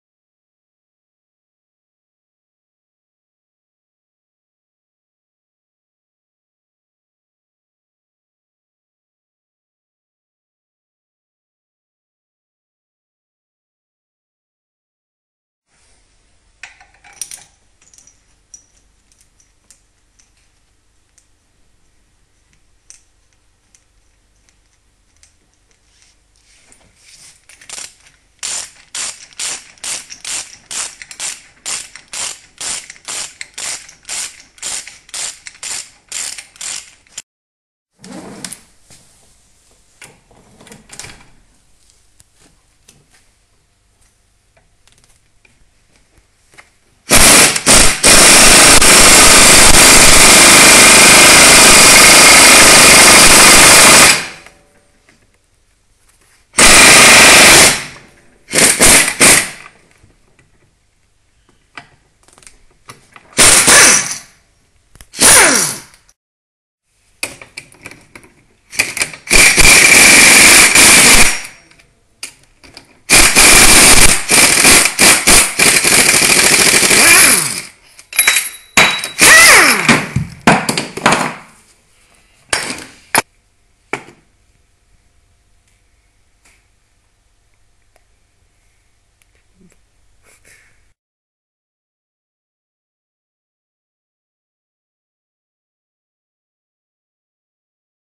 Gelenkwellenabzieher Werkzeug Demontage Gelenkwelle Abzieher
Das äußere Antriebswellengelenk lässt sich nicht mehr einfach lösen, da sich der Sicherungsring im Inneren durch abgeriebene Metallspäne verkantet hat. Die Demontage erfolgt dann über den Gelenkwellenabzieher, der die nötige Kraft aufbringen kann das Gelenk trotz des defekten Sicherungsrings abzuziehen. Spezialwerkzeug von XXL-Automotive: